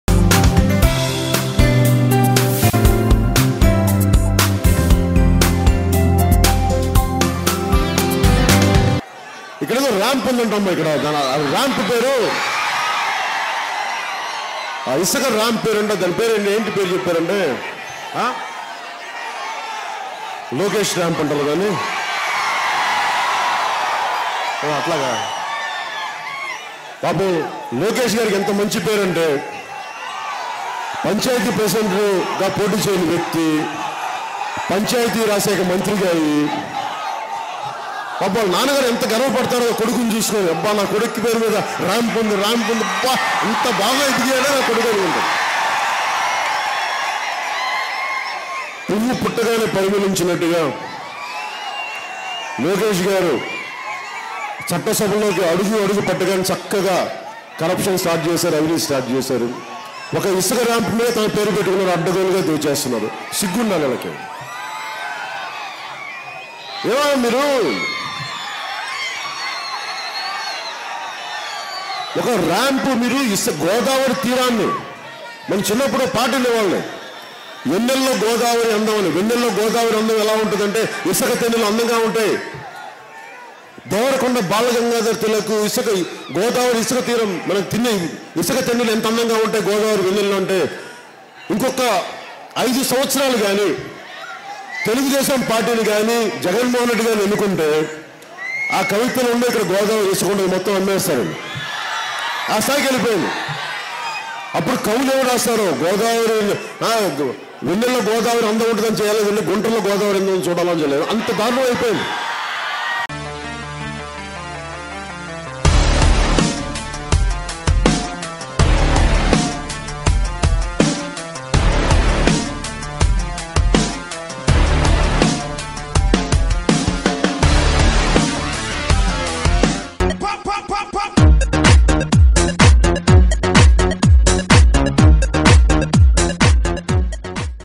లోకేష్ ను ఇమిటేట్ చేస్తూ పవన్ కల్యాణ్ సెటైర్లు| Pawan satires on Minister Lokesh over Sand Mafia
Janasena Party Chief Pawan Kalyan has made satirical comments on Minister Nara Lokesh over Godavari Sand Mafia.
Watch : Pawan Kalyan speech
Amaravathi Media